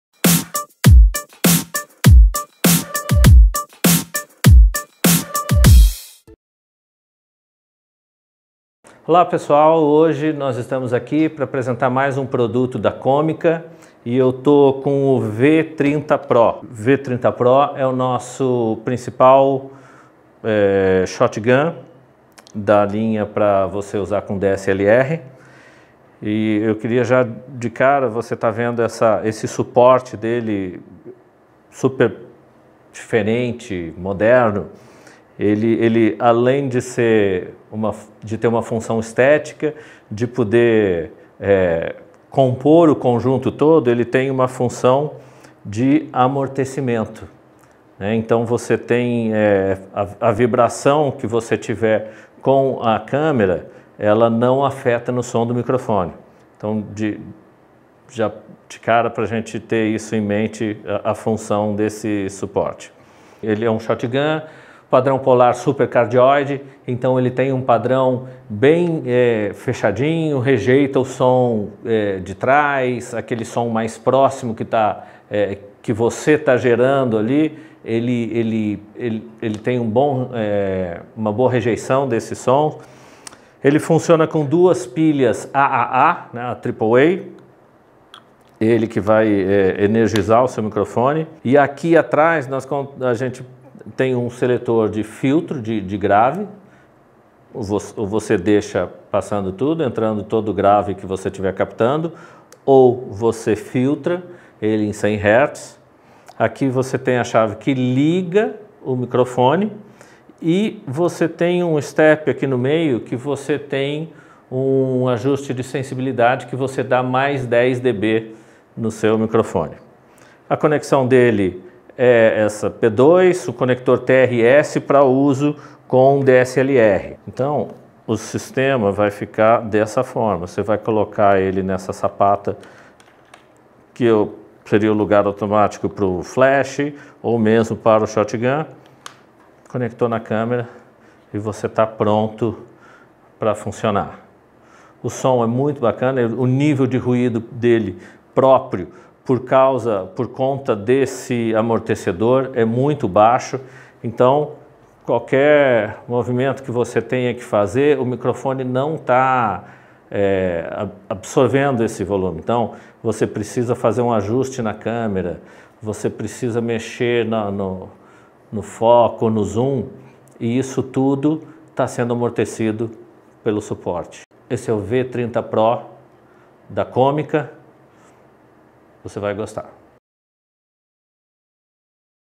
Comica V30 Pro - Shotgun para DSLR
A Seegma Pro Áudio está trazendo para o mercado os produtos da Comica, uma linha de produtos de alta qualidade, com opções excelentes de custo muito acessível para todos que se depararam com a necessidade de fazer lives ou melhorar a qualidade de captação.

Neste vídeo apresentamos um microfone de lapela para smartphone, o Comica V30 Pro, Shotgun para sua DSLR.